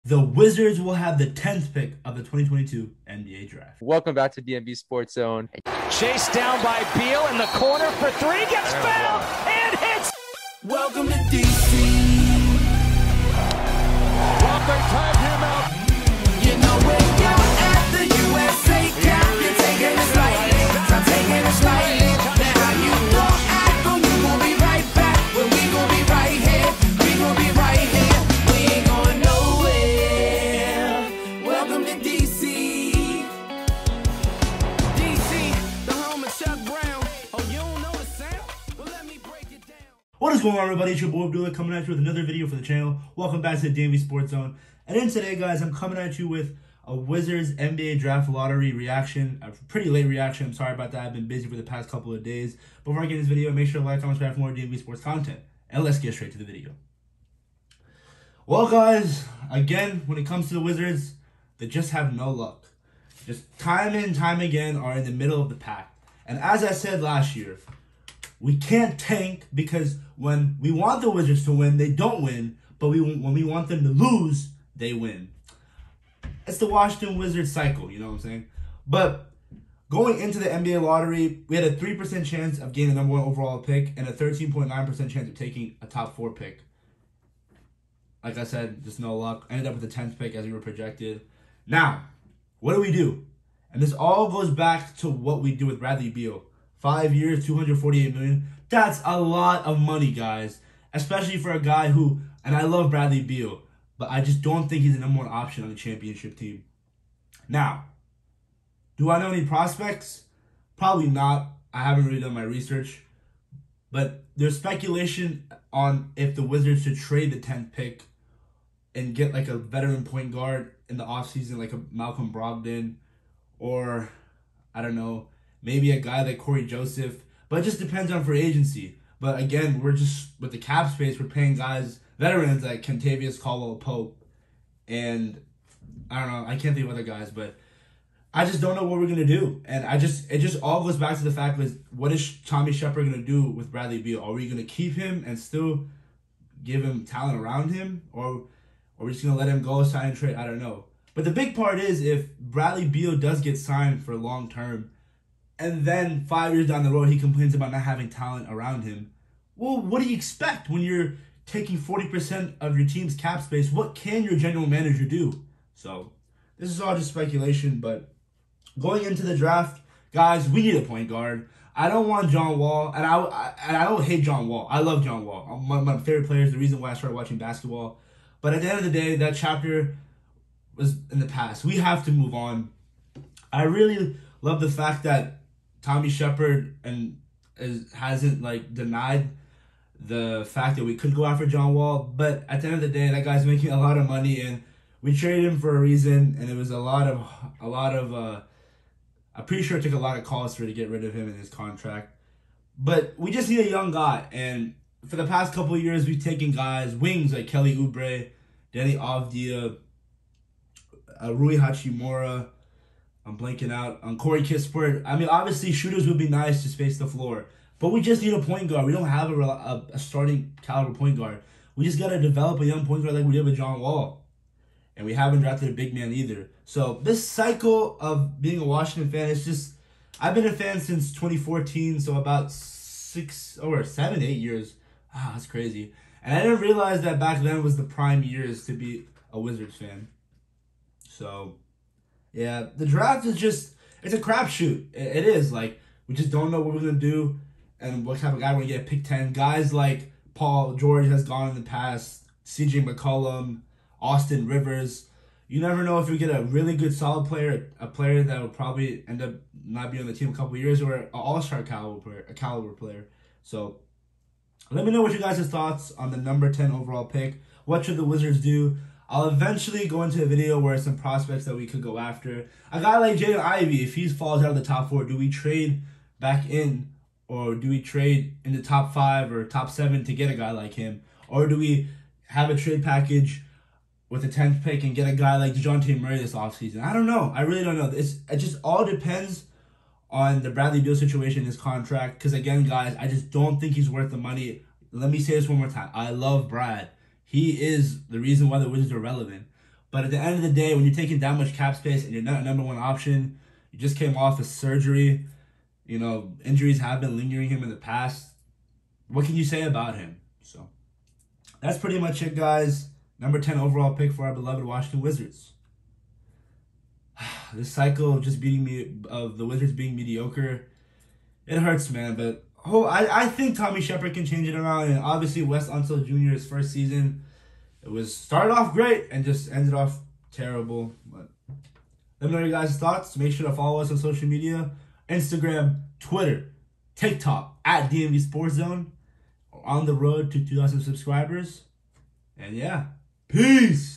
Wizards NBA Draft Lottery Pick Reaction! What Do We Do At Pick 10? Is Bradley Beal Staying?
Follow DMVSportsZone